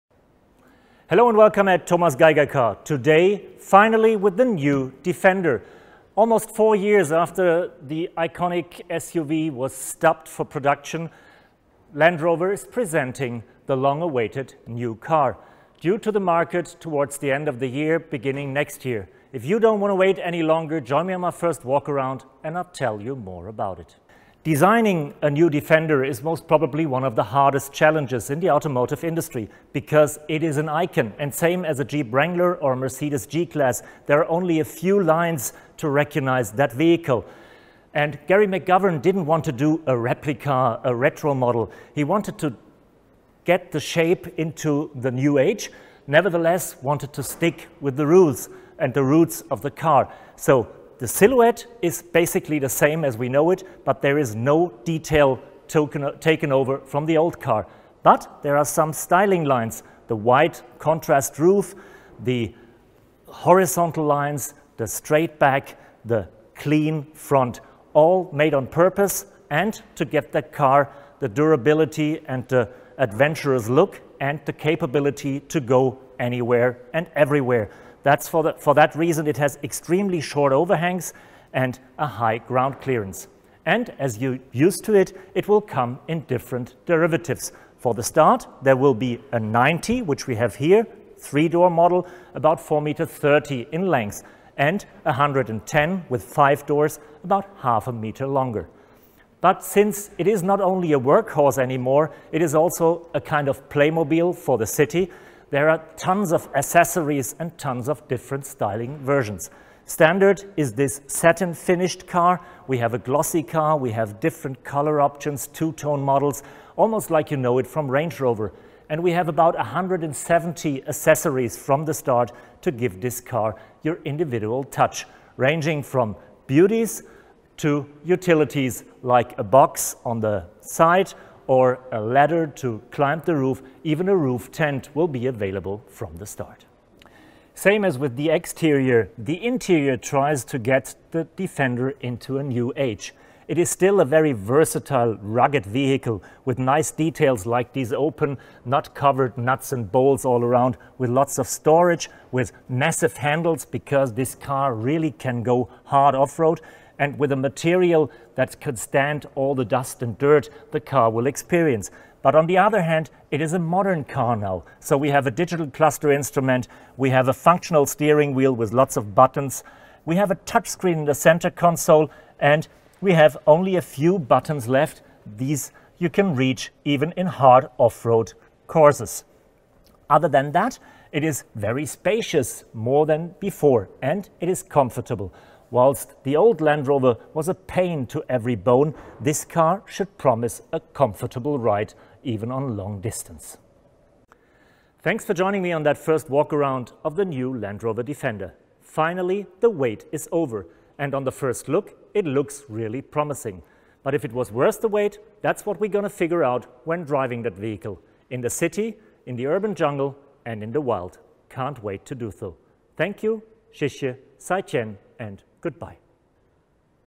2021 Land Rover Defender - First Video Walk Around
Finally, it's back. Land Rover is revealing the successor of the iconic Defender and we have the first insights. Still a legend? Still a tool or more a toy? See for yourself.